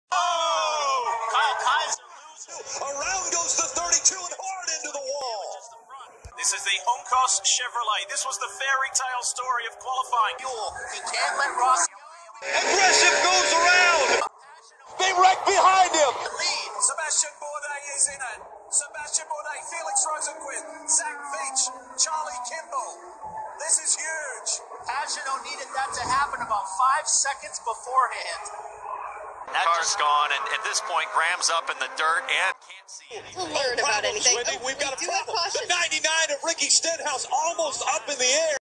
Indy 500 clips being commentated by Rick Allen
Something funny I just thought of...